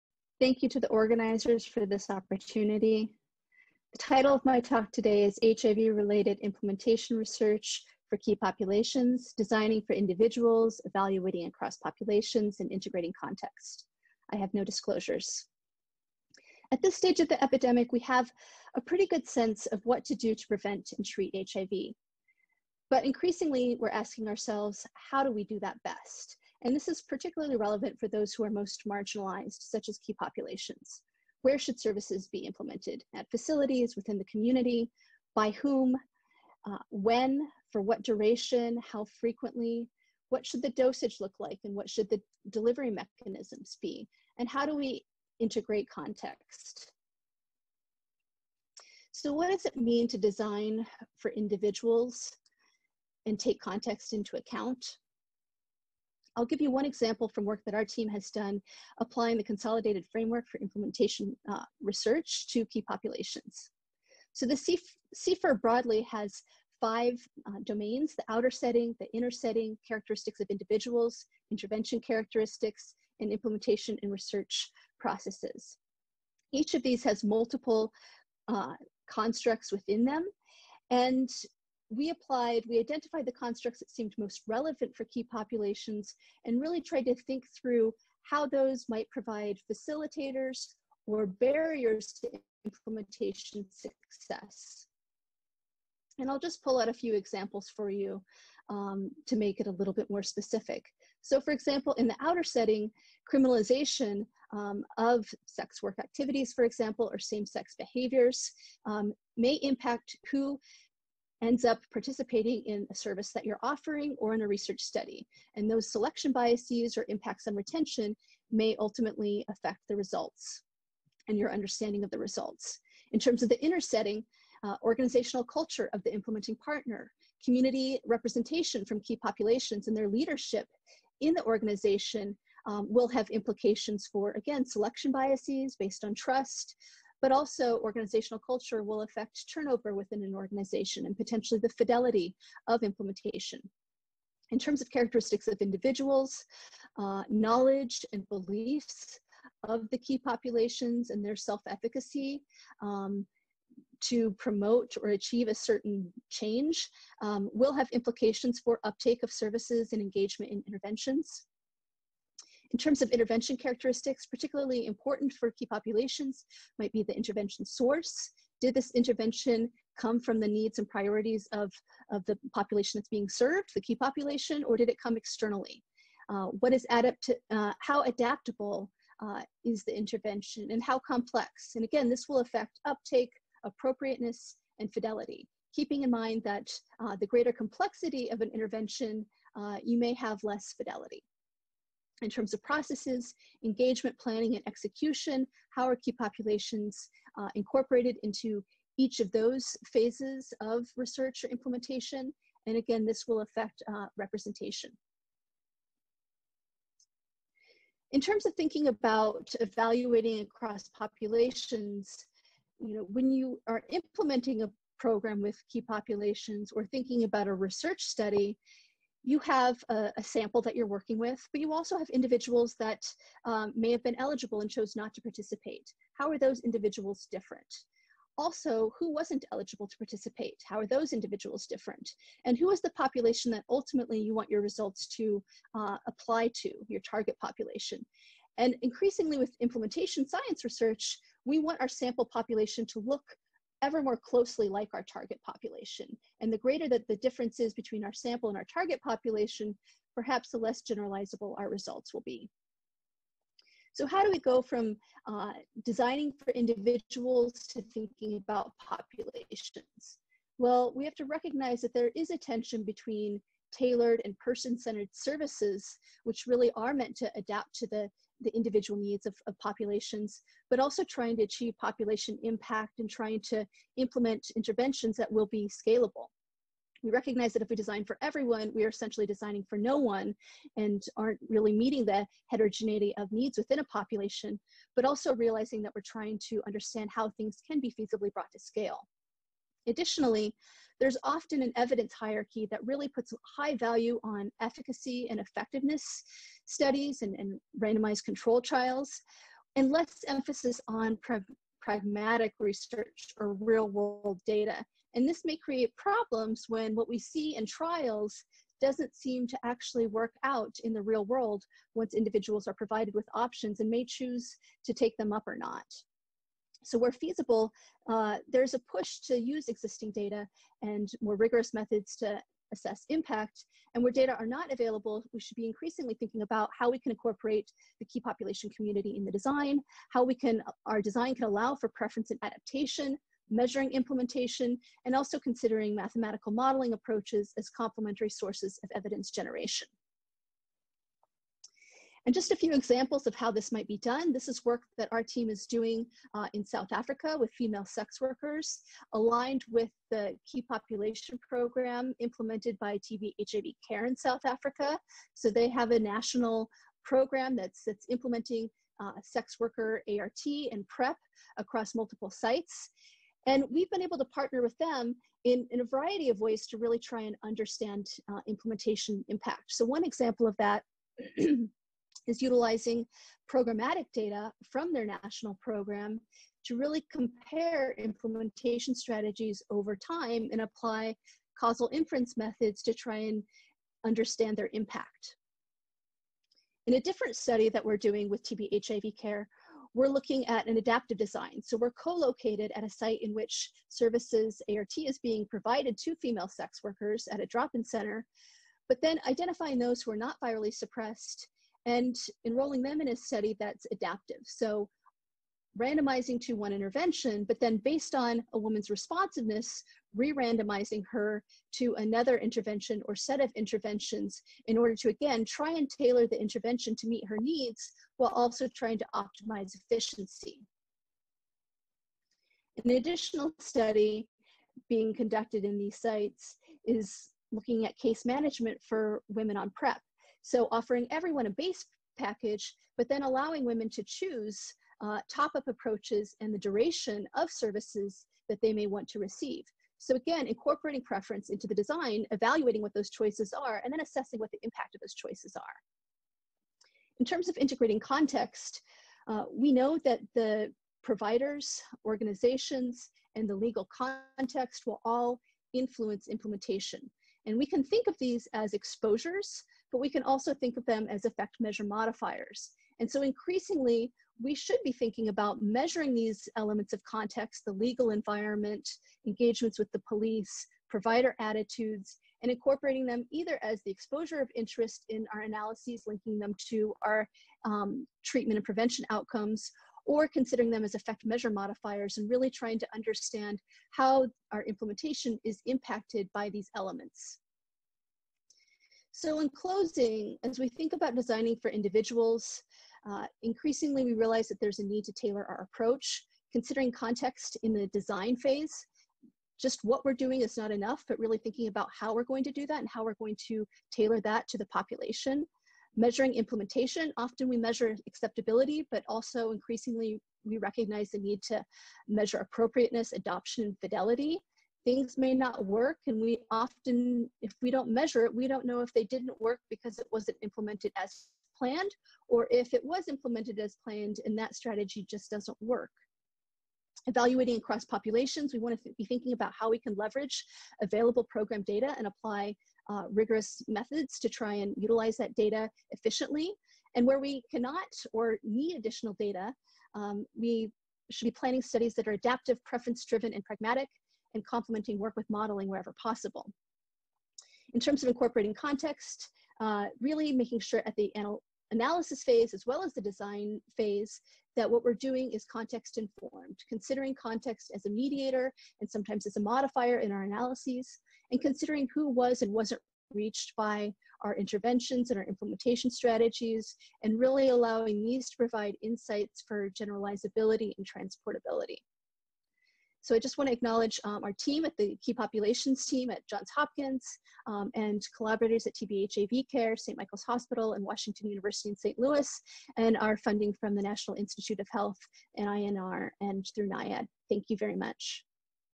Исследования по вопросам осуществления мер борьбы с ВИЧ среди основных групп населения
выступающая: Sheree Schwartz. Презентация 23-й Международной конференции по СПИДу (AIDS 2020: Virtual). Узнайте подробнее на